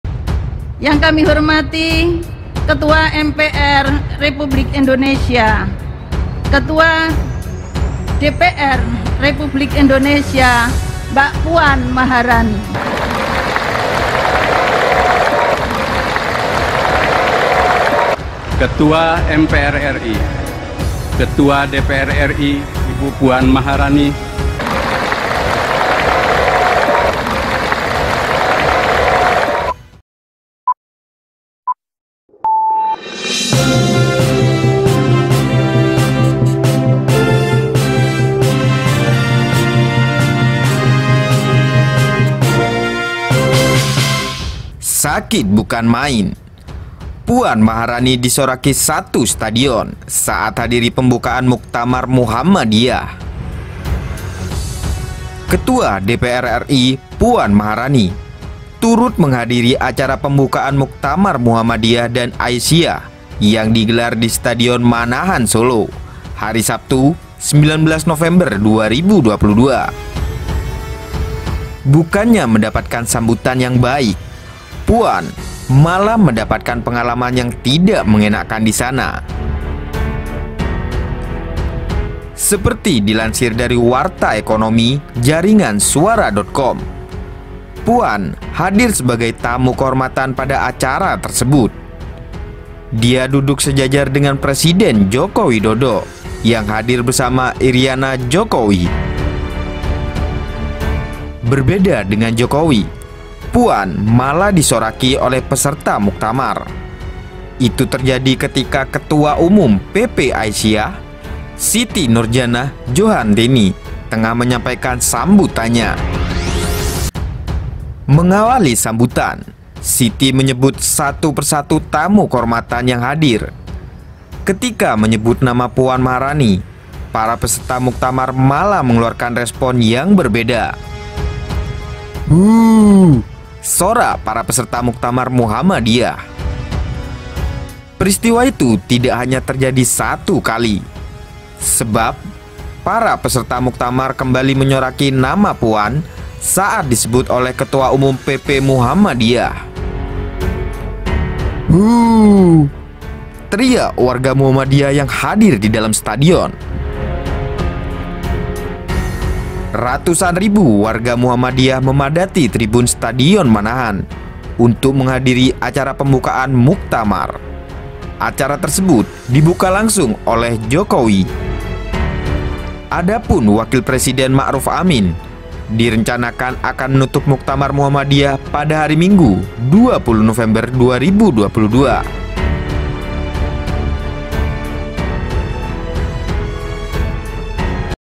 NEWS.HADIRI PEMBUKAAN MUKTAMAR MUHAMMADIYAH, PUAN DISORAKI SATU STADION..!!!
news news90s puanmaharani ~ Ketua DPR RI Puan Maharani turut menghadiri acara Pembukaan Muktamar Muhammadiyah dan Aisyiyah yang digelar di Stadion Manahan Solo, Sabtu (19/11/2022). Bukannya mendapatkan sambutan yang baik, Puan malah mendapatkan pengalaman yang tidak mengenakkan di sana.

Seperti dilansir dari Warta Ekonomi--Jaringan Suara.com, Puan hadir sebagai tamu kehormatan pada acara tersebut. Ia duduk sejajar dengan Presiden Joko Widodo atau Jokowi yang hadir bersama Iriana Jokowi.

Berbeda dengan Jokowi, Puan malah disoraki oleh peserta Muktamar. Itu terjadi ketika Ketua Umum PP Aisyiyah, Siti Noordjannah Djohantini tengah menyampaikan sambutannya.